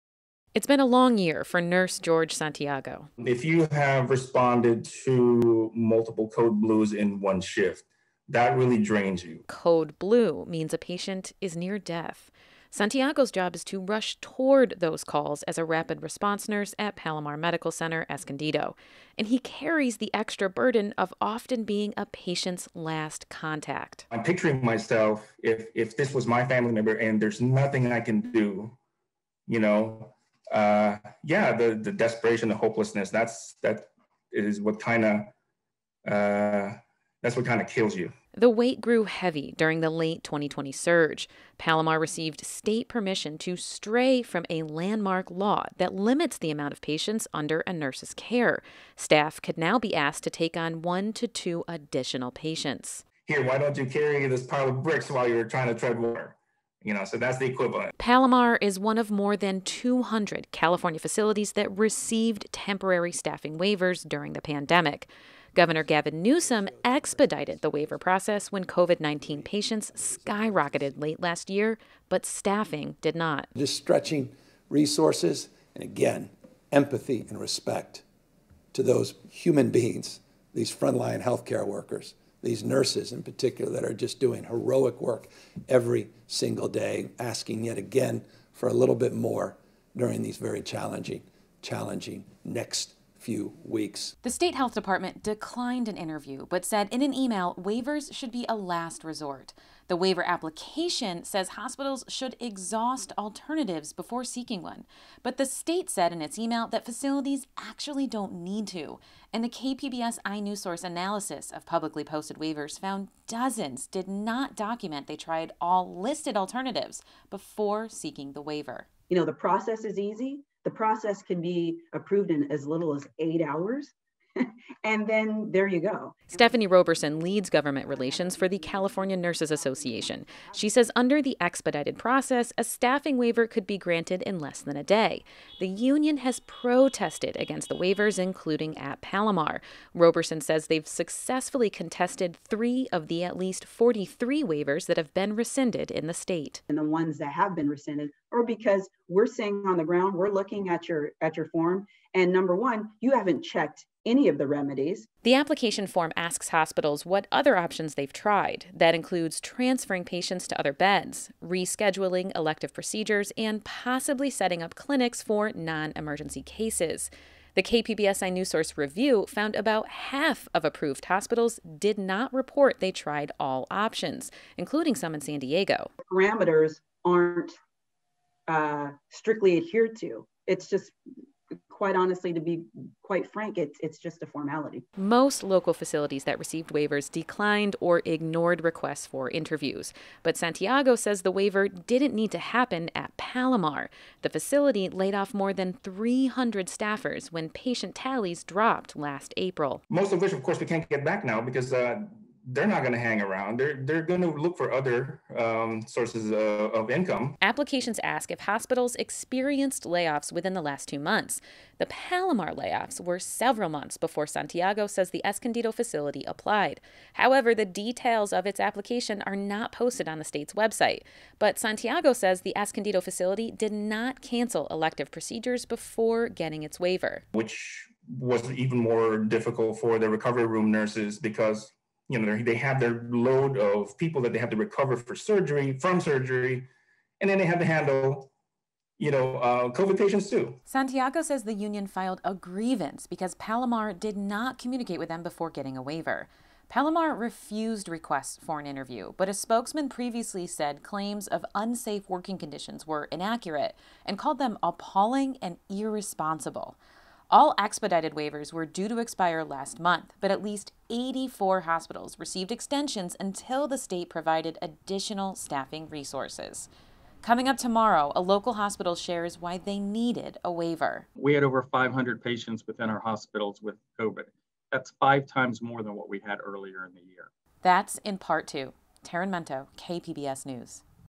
California Hospitals Given Leeway To Use ‘Last Resort’ Staffing Waivers, Analysis Shows
California saw hospitalizations skyrocket around the holidays. And when staffing couldn’t keep up...the state let hospitals put more patients on a nurse’s workload than law usually allows. In the first of a two-part series… KPBS Health Reporter Tarryn Mento tells us how nurses felt pushed to the brink by the move and questioned whether it was even necessary.